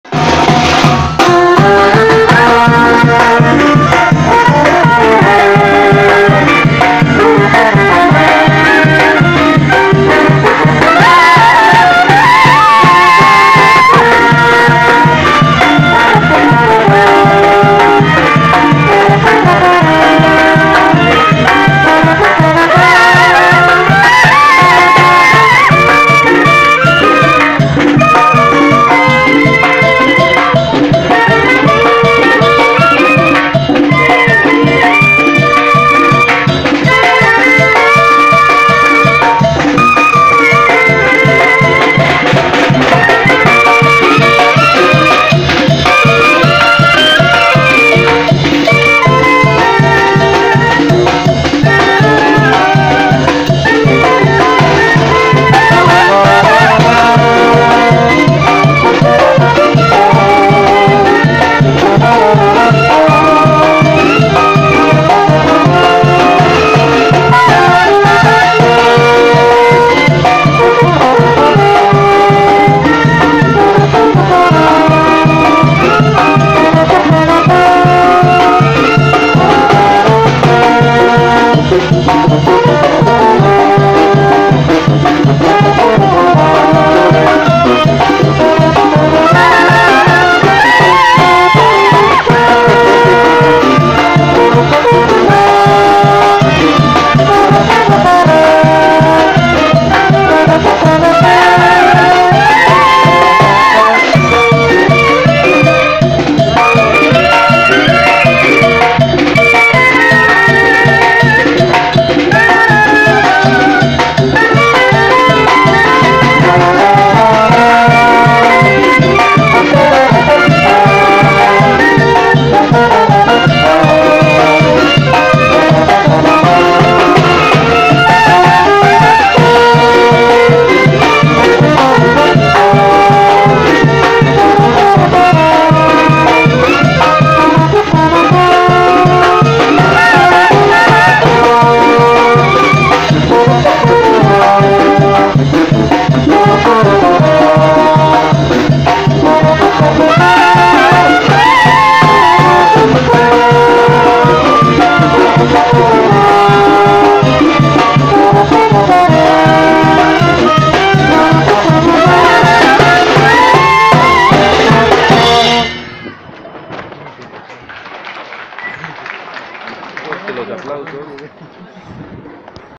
Festejo a la virgen de la Asunción teococuilco
Mayordomo de la virgen de la asunción en teococuilco de Marcos Perez 2018